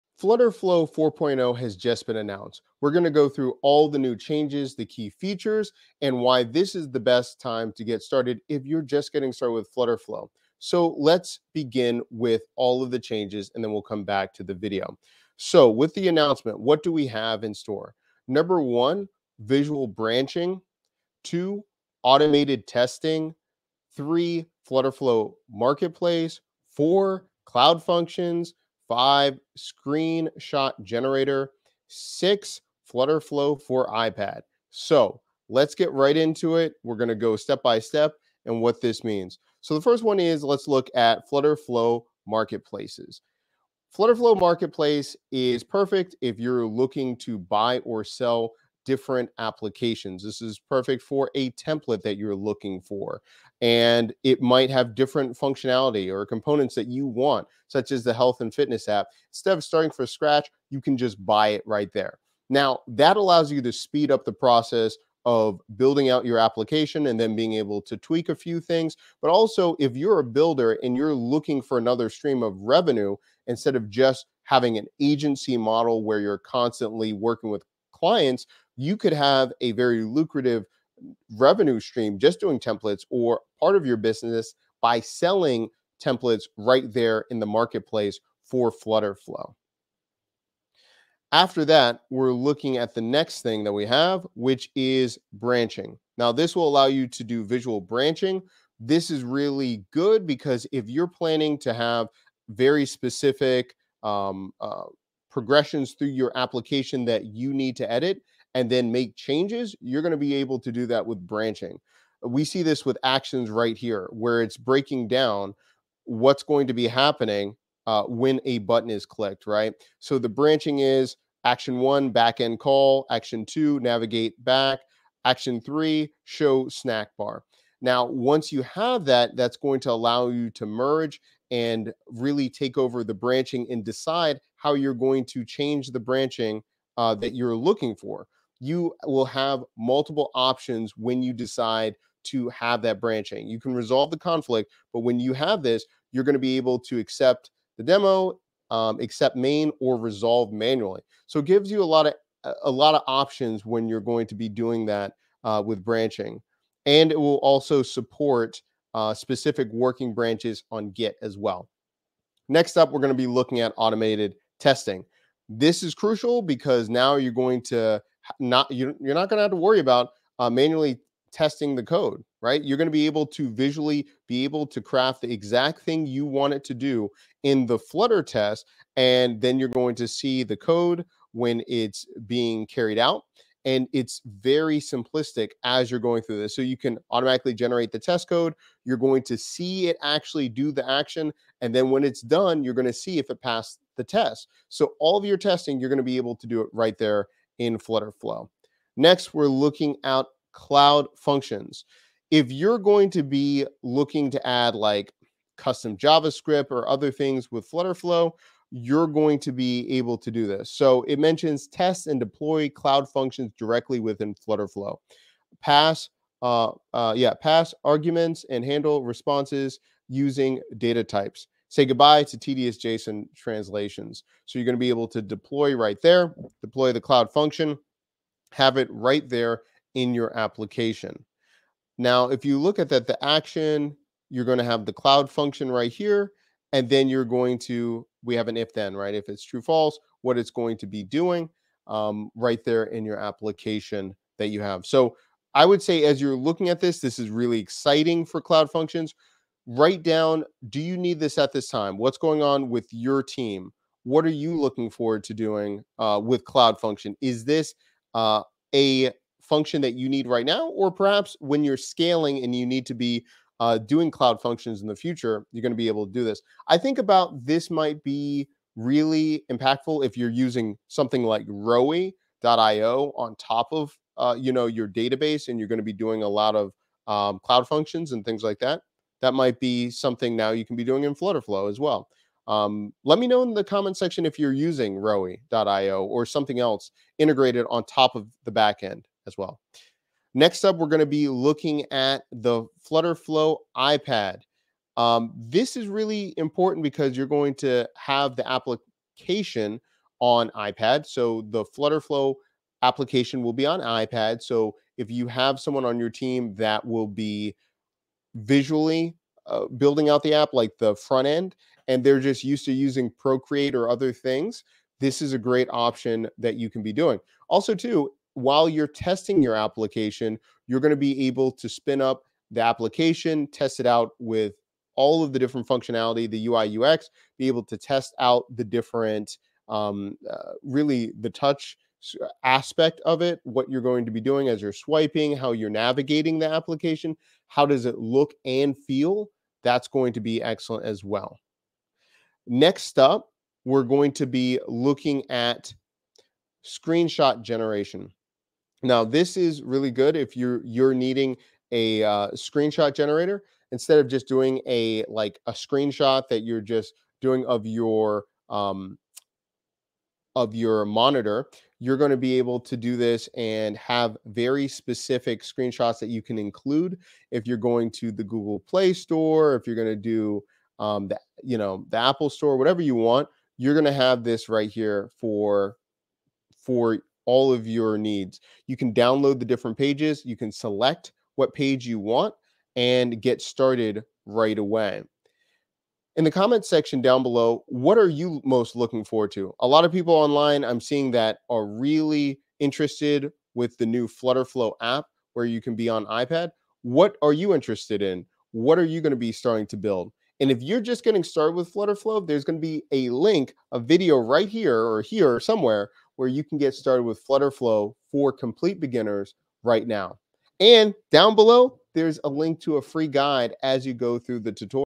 FlutterFlow 4.0 RELEASED | 6 NEW Features that are Game Changers
0:43 FlutterFlow Marketplace: Buy or sell templates
1:35 Visual Branching: Experience git-style branching
2:42 Automated Testing
3:19 Cloud Functions: Design and deploy cloud functions directly within FlutterFlow.
4:54 FlutterFlow iPad app
5:43 Screenshot Generator: Auto-generate screenshots for all devices

Tips and Tools for Building a Successful Business
50+ High-Profit Side Hustles 🤑